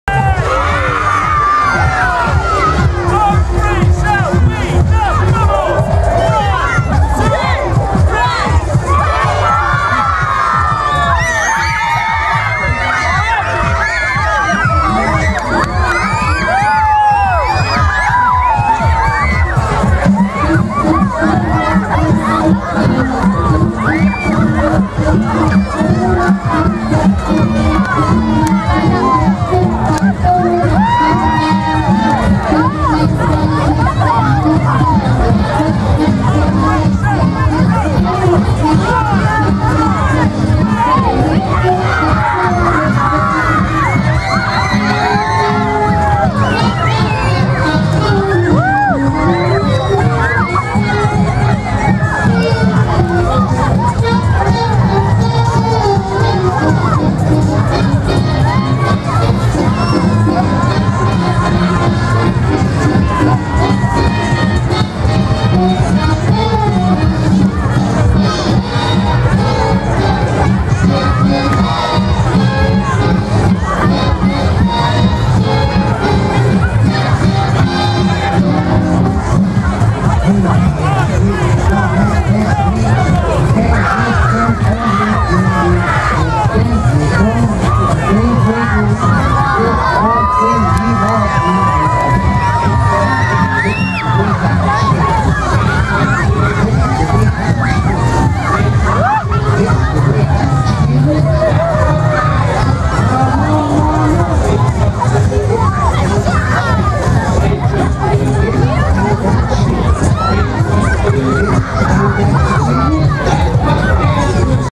The Bubble Wizard at Blackheath Festival Sept 2014
The Bubble Wizard entertaining the festival lovers at the Blackheath festival Sept 2014.